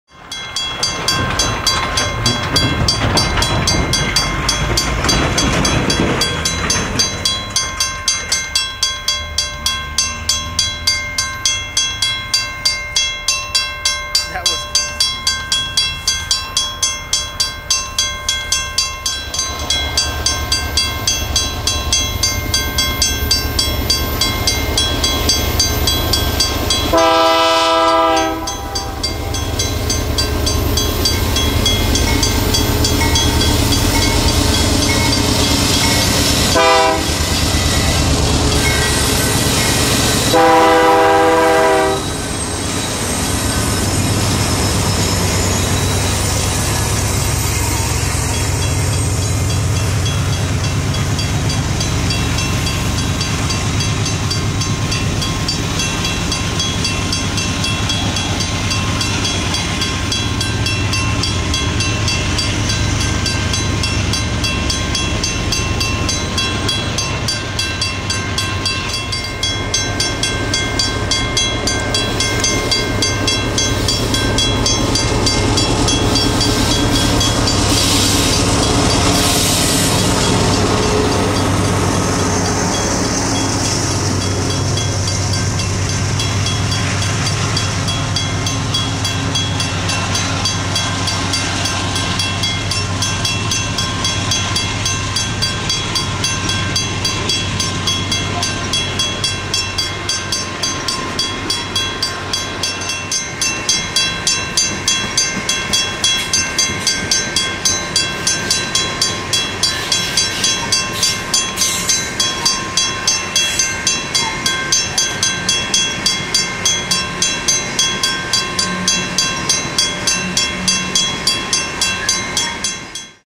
CP9603 West - Notch Hill, B.C.
Nearing the summit of the climb, CP9603 and CP9662, with CP9665 mid-train, grind up the north track at Notch Hill, British Columbia at 17:55 on August 7, 2001 with a westbound coal train.